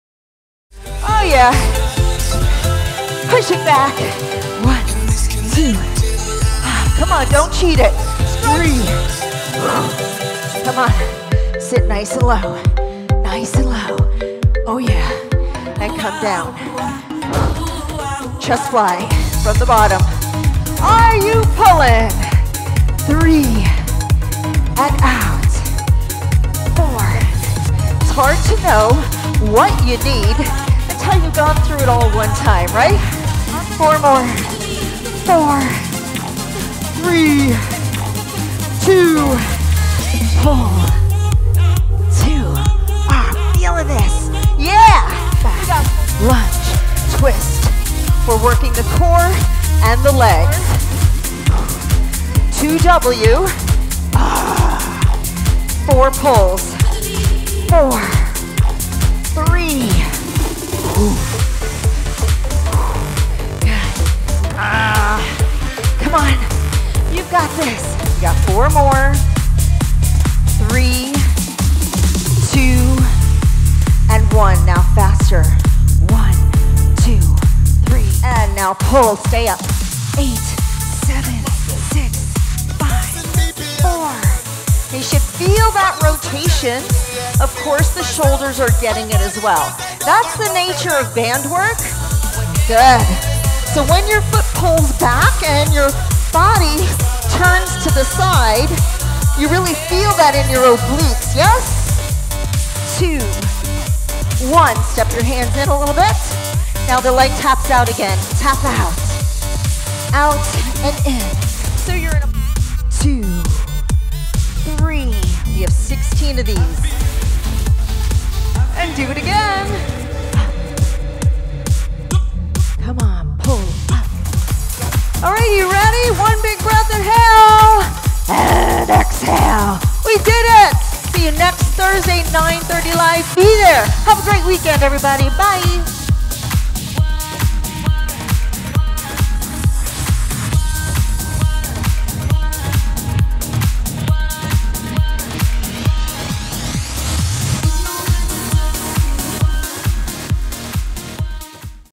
Cathe Friedrich's Fantastic Elastic Live Workout
Attention Cathe Live subscribers: Thursday, Nov 12, 2020, at 9:30 am EST we’ll be doing “Fantastic Elastic” LIVE.  A total body elastic workout using a light tension resistance tube and a medium tension fitness loop (pictured below).  This lighter workout is perfect for days where you want to challenge your muscles without lifting heavy weights.  It’s also a perfect workout to travel with.  Let’s dive right into this high rep lighter resistance workout!  Side Note:  If you want the workout to be harder, use a medium tension resistance tube instead of a light one).

Equipment Needed:
A light tension resistance tube (I’ll be using my new red Cathe band)
A medium tension fitness loop (I’ll be using my new green Cathe loop)
A fitness mat (and yes, you guessed it, lol, I’ll be using my new Cathe aqua mat). a fitness guru can ever have enough fitness toys

✅Over 300 premium workouts
✅ 322+ Cathe Live workouts
✅ 3300+ premix workouts
✅ 150+ workout rotations
✅ Create Your Perfect Workout With Our Workout Blender